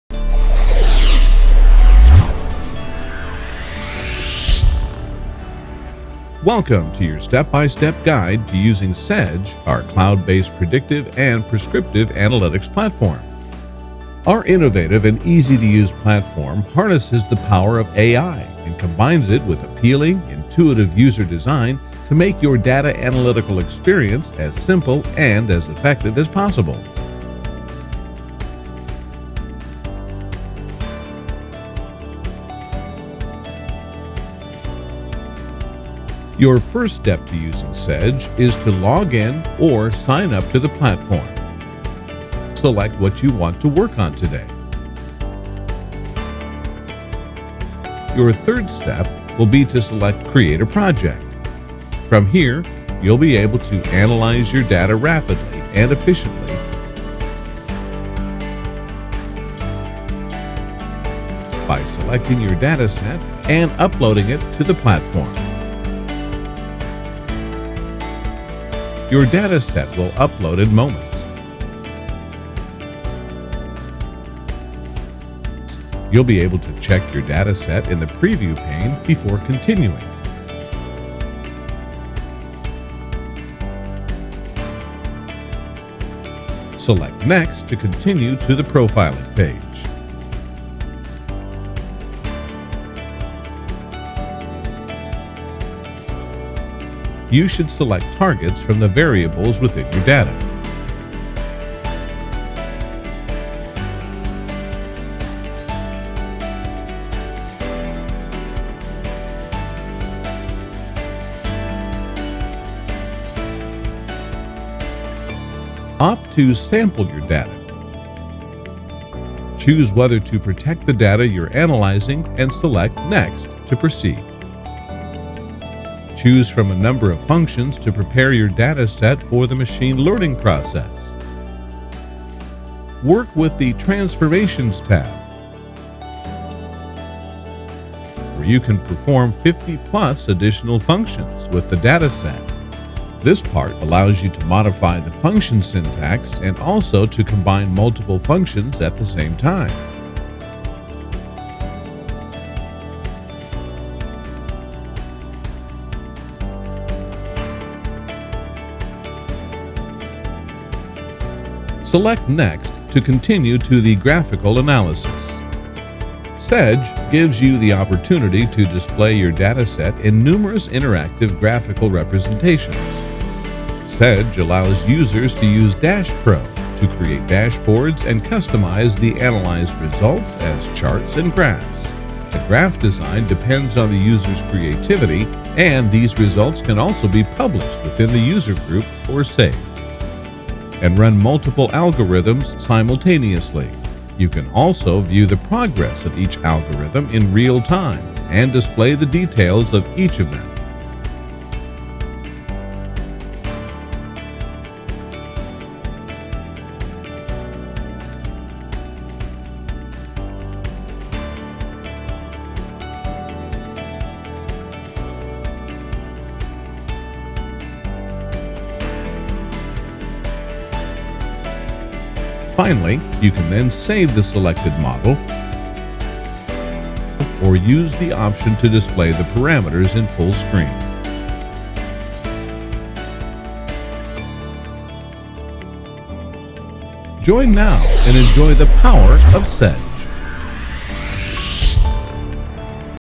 SEDGE | Our step-by-step guide
Watch our step-by-step DEMO guide to using SEDGE. Learn how to effortlessly create projects, upload datasets, use multiple algorithms to build models and graphical analysis to analyze your data quickly and effectively.